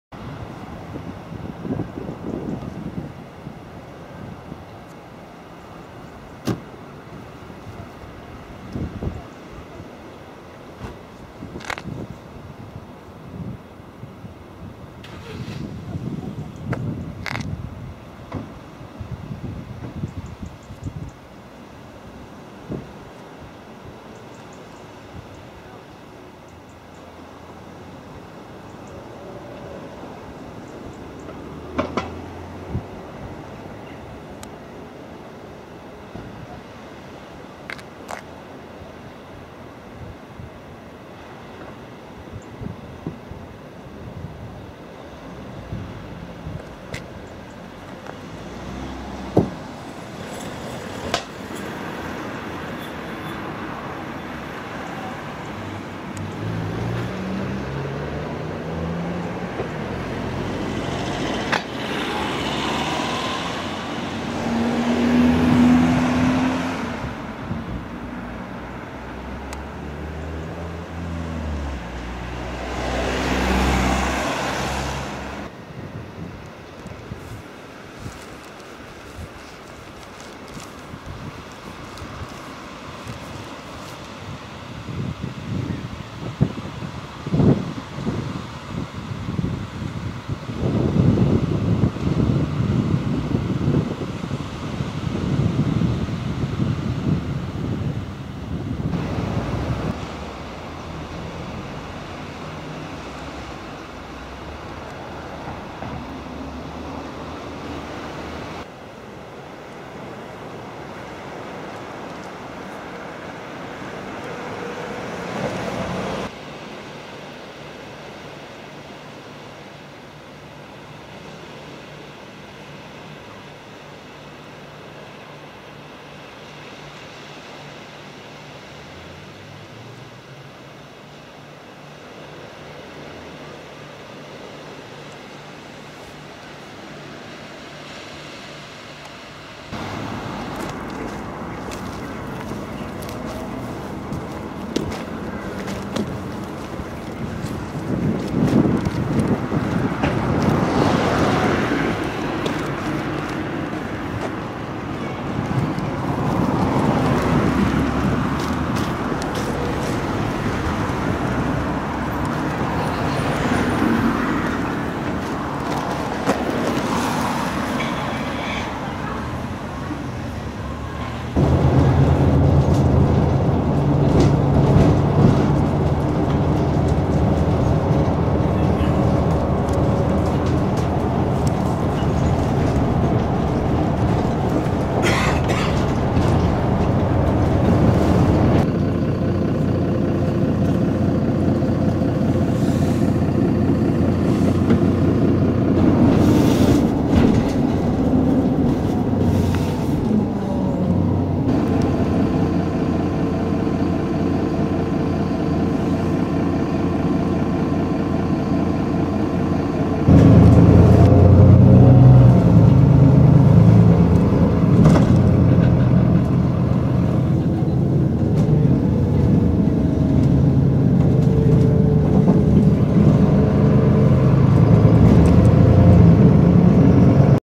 Port Erin town-Isle Of Man 06/09/2012
Last steam train from Douglas to Port Erin,after a brief look around the town,a drink of Okells and Erin tasty fish & chips,caught the bus back,which was bright and comfortable.